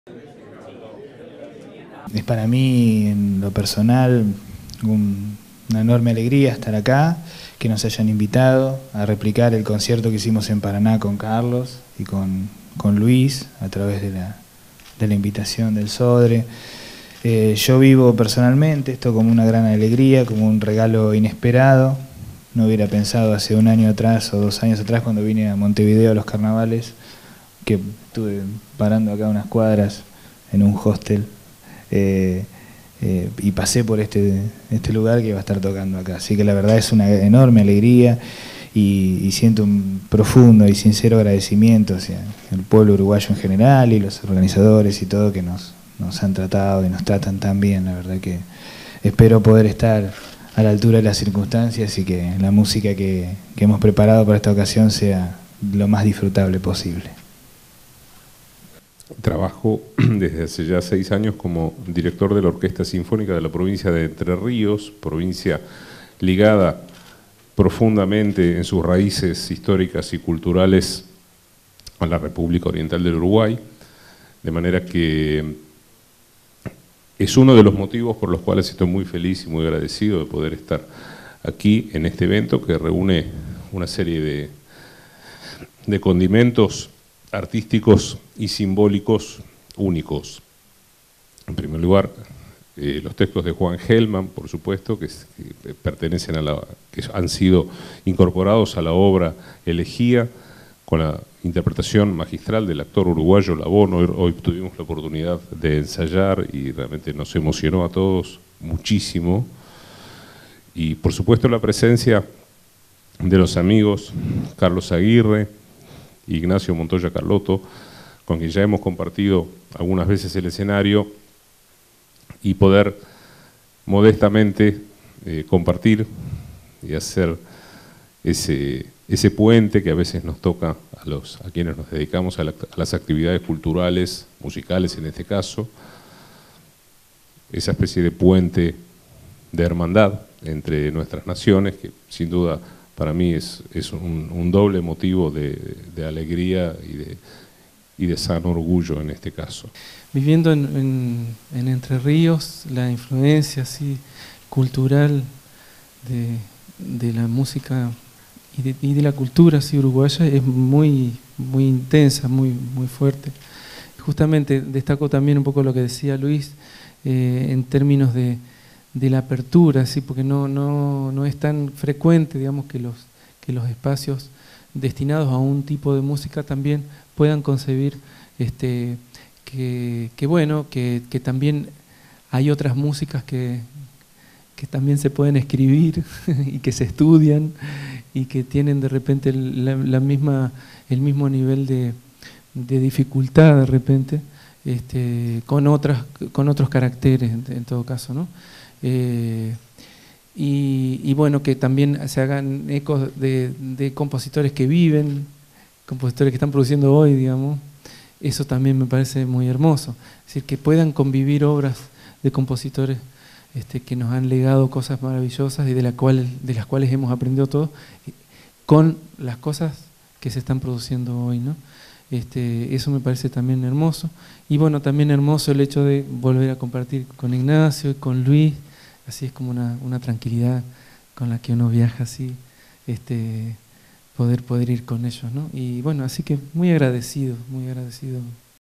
Conferencia de prensa sobre el Concierto Por la Memoria
Ariel Cazes, director artístico del Sodre, presentó a los medios de comunicación el concierto Por la Memoria, un ensamble de destacados músicos argentinos con la Orquesta Sinfónica del Sodre que interpretan obras contemporáneas.

En la presentación, además del Consejo directivo del Sodre, estuvieron el director del concierto Luis Gorelik y dos de los protagonistas, Ignacio Montoya Carlotto y Carlos 'Negro Aguirre'.

Auditorio Nacional del Sodre. Montevideo, 18 de febrero de 2015.